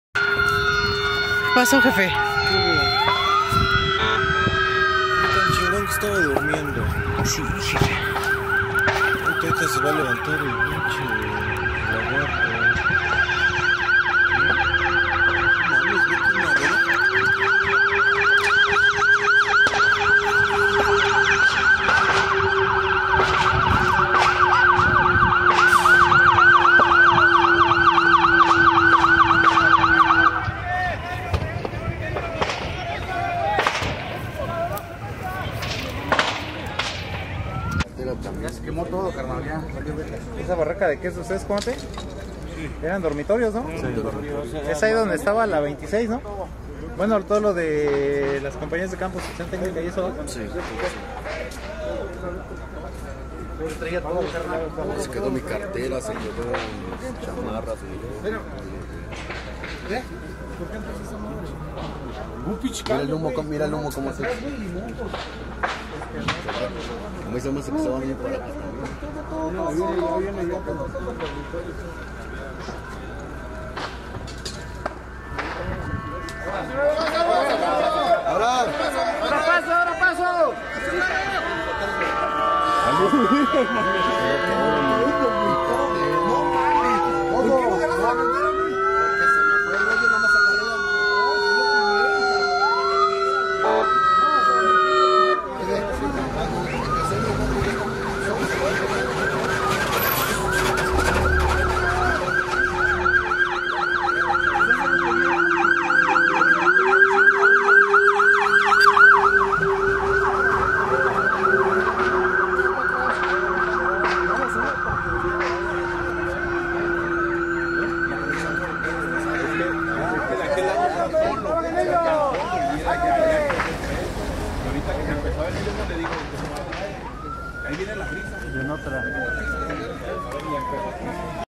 Incendio consume instalaciones de la Guardia Nacional CONTEL
La noche de este lunes se registró un fuerte incendio en las instalaciones del Centro de Mando de la Guardia Nacional ubicadas en la Alcaldía Iztapalapa, en la zona de dormitorios